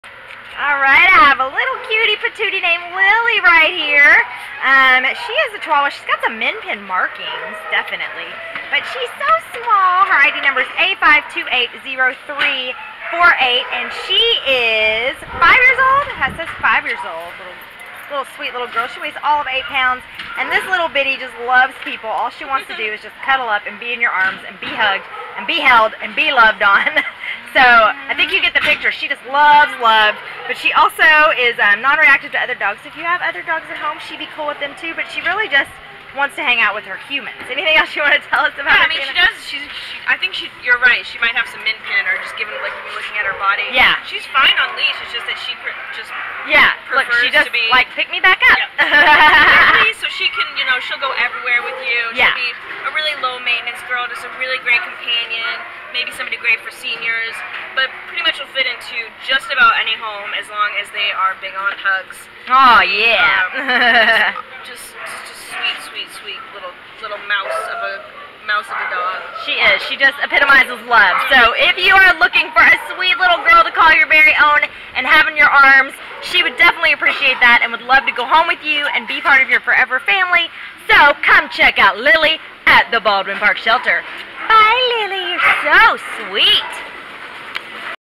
A5280348 Lilly | Chihuahua / Miniature Pinscher
A5280348 Lilly is a perfect 5-year-old chocolate and vanilla Chihuahua and Miniature Pinscher mix who was found as a stray in Azusa and brought to the Baldwin Park Animal Care Center on May 16th. Weighing 8 lbs, Lilly is a go along and get along kind of girl who will fit in just about anywhere. She walks fine on leash but prefers to be held and carried so she can see what’s happening around her. Lilly loves to meet new people but she isn’t as interested in other dogs as she is in snuggles and pets from her handler. With her petite princess size, she could be a happy “purse pet” to go along on outings, or she would be content with a person who likes to spend a lot of time at home too. Lilly will be a happy companion in any home where she gets the attention and love she craves.